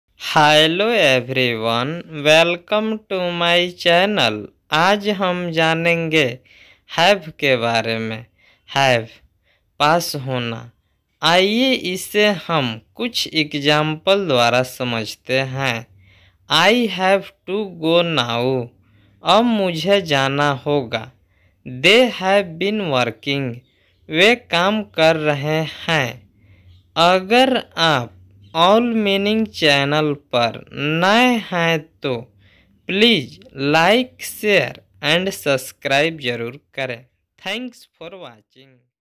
Have meaning in Hindi | Have ka matlab kya hota hai | Explained Have in Hindi
Have meaning in Hindi
Have ka matlab kya hota hai
Explained Have in Hindi

इस वीडियों में आप Have का हिंदी में मतलब समझेंगे और साथ में Have का Pronunciation भी सीखेंगें यानि कि आप Have के अर्थ के साथ-साथ ये भी सीखेंगें की Have कैसे बोलें या Have बोलने का सही तरीका क्या हैं|

Have का हिंदी में अर्थ होता हैं|

( पास होना )

Have को इन उदाहरणों से और अच्छे से समझ सकते हैं|

Exp - I have to go now.
          अब मुझे जाना होगा|

          They have been working.
          वे काम कर रहे हैं|

अगर आप हमारी इसी तरह की शिक्षा प्रद जानकारियाॅं पाते रहना चाहते हैं तो हमें फॉलो जरुर कीजिए|